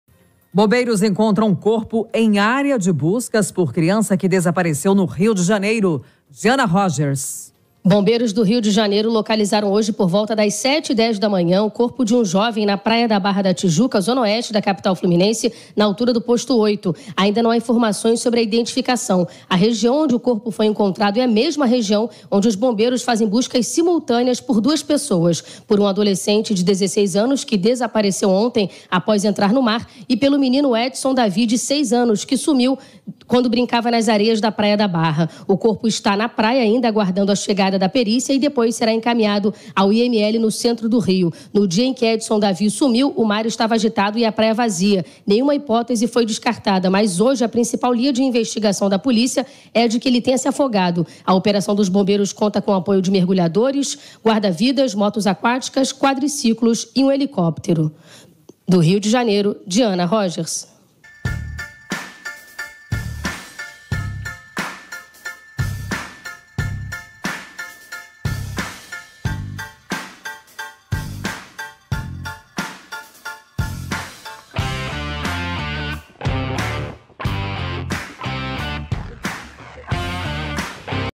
URGENTE! BOMBEIROS ENCONTRAM CORPO DE JOVEM EM ÁREA DE BUSCA POR EDSON DAVID
Militares do Corpo de Bombeiros do Rio de Janeiro confirmaram que encontraram um corpo dentro da área de busca por Edson David, menino de 6 anos que desapareceu na Praia da Barra da Tijuca, na última quinta-feira (4). Até o momento, não há informações sobre a identidade. Na região, uma segunda pessoa também está sendo procurada.